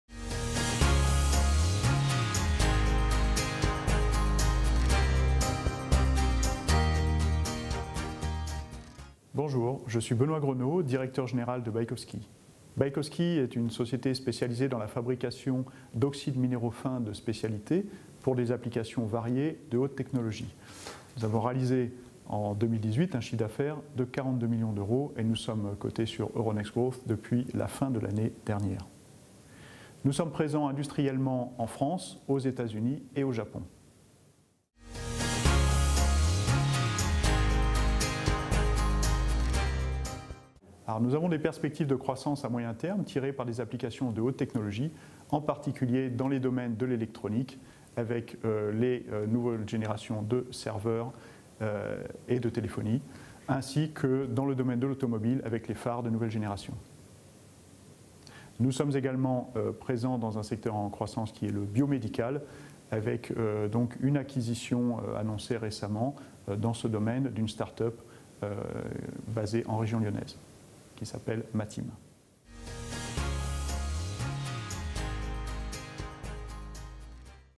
Interview de Benoît Grenot, PDG de Baikowski
Cet entretien a été réalisé lors du second forum de Lyon Pôle Bourse des Valeurs Régionales en septembre 2019, qui a réuni 38 sociétés cotées emblématiques et implantées dans les régions Auvergne-Rhône-Alpes et Bourgogne-Franche-Comté, et plus de 70 investisseurs professionnels et analystes financiers. Baikoski opère en tant que fabricant de spécialités inorganiques. L'entreprise produit et distribue des poudres pour les marchés de l’éclairage, l’électronique, l’automobile, l’aéronautique & la défense, le médical, l’horlogerie & les smartphones.